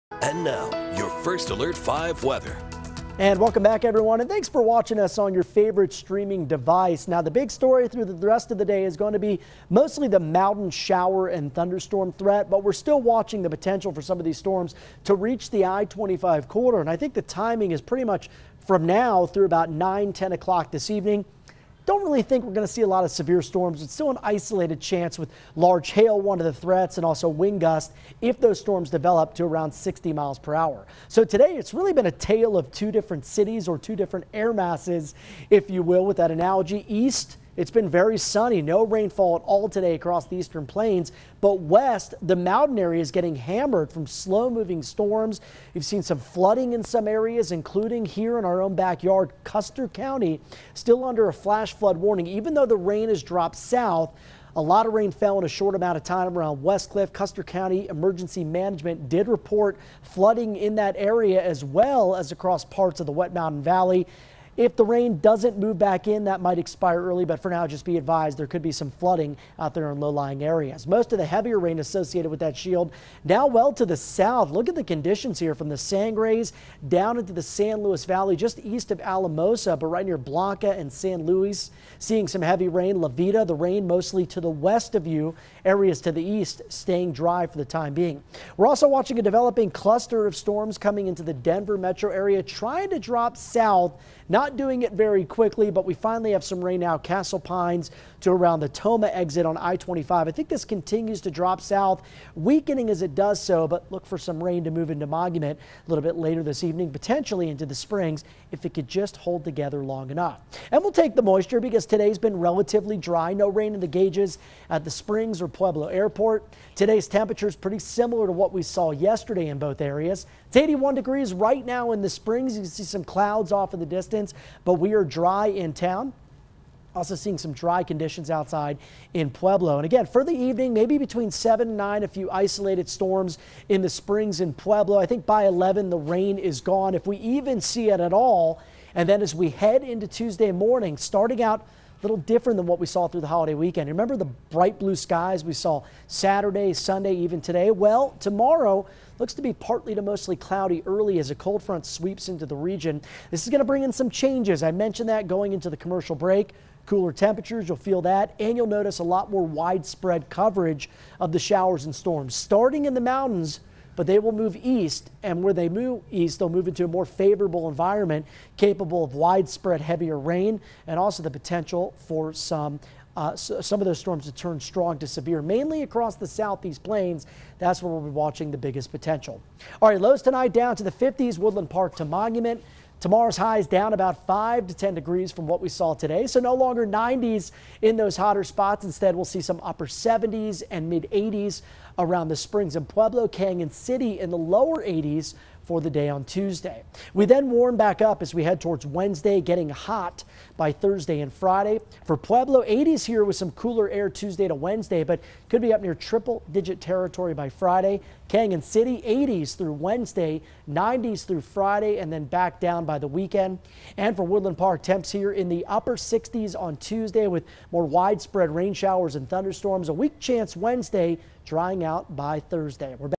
Widespread storms Tuesday, with increased heavy rain threat
A cold front will bring better chances for showers and thunderstorms to our forecast on Tuesday, with both a heavy rain and severe weather threat likely.